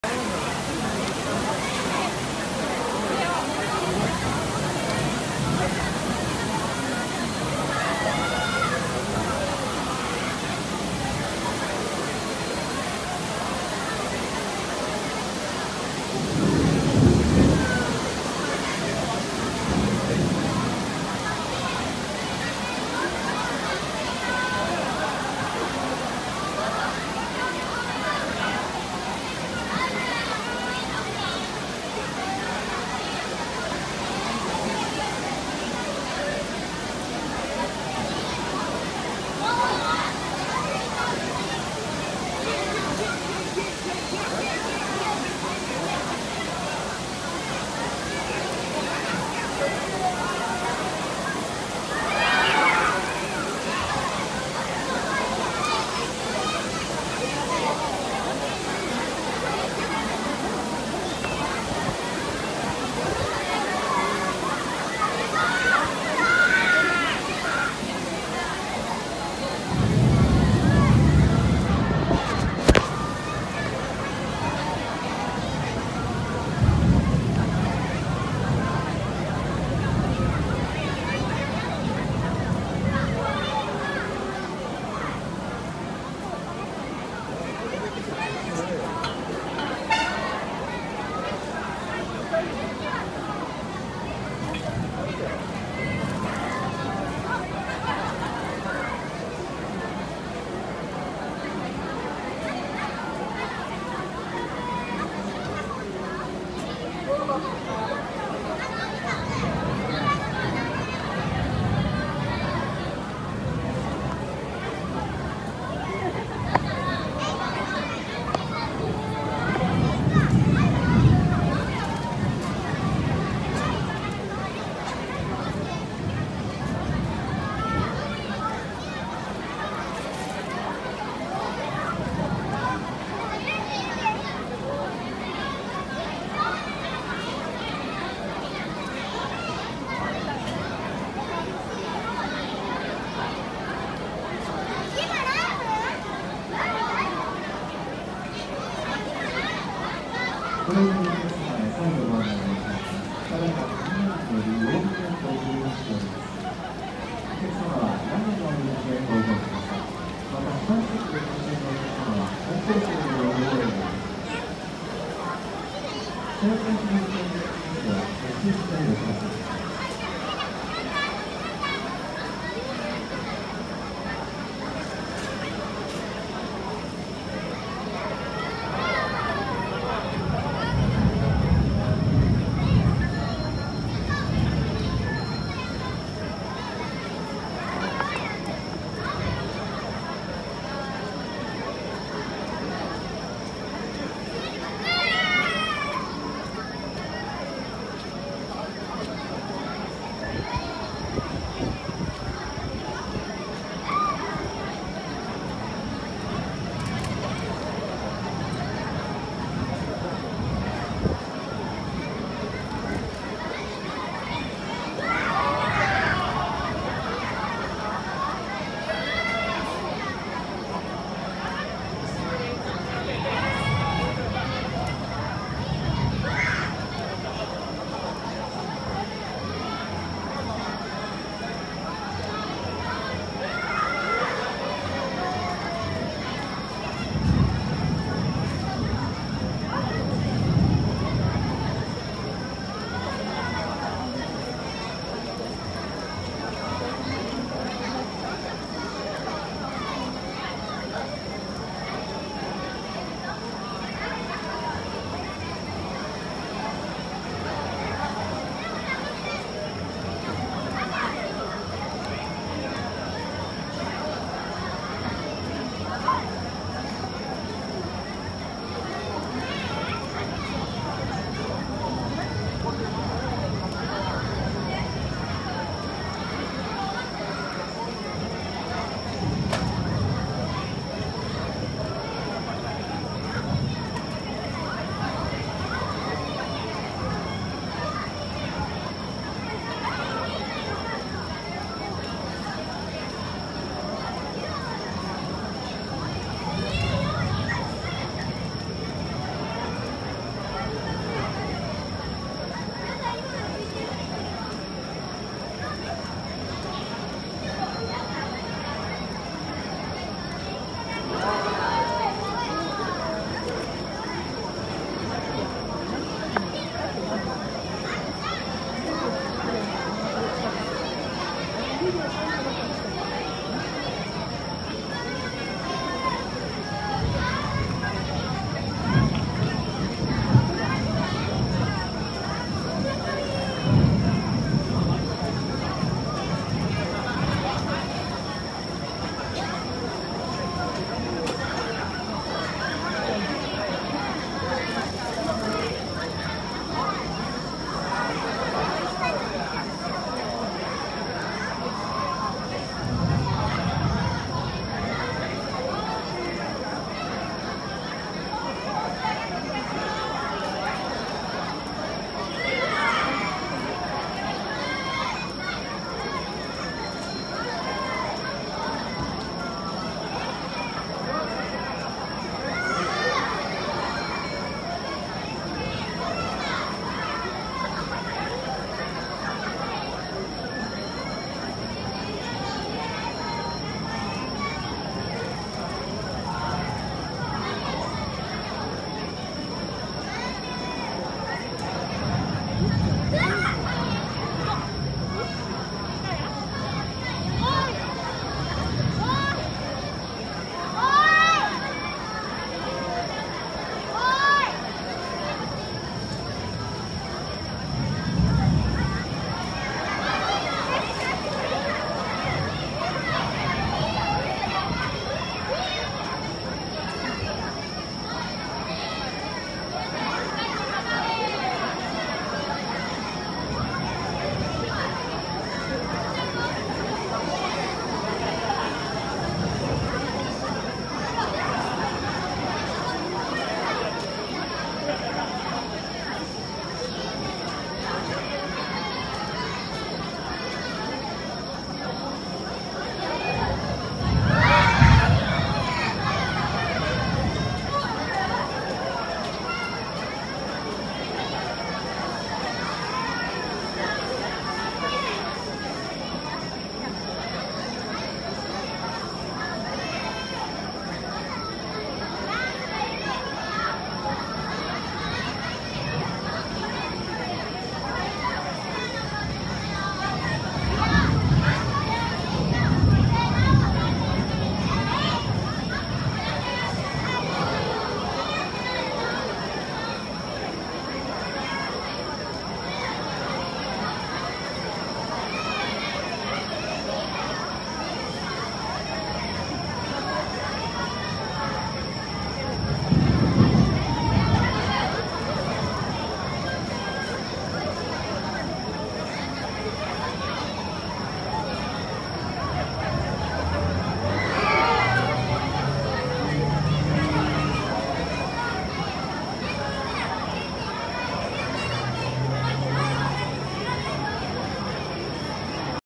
2023/08/26 ザスパ 藤枝の雨
ウォーミングアップ中に雨が降り、
一旦スタジアムの裏に集まりました。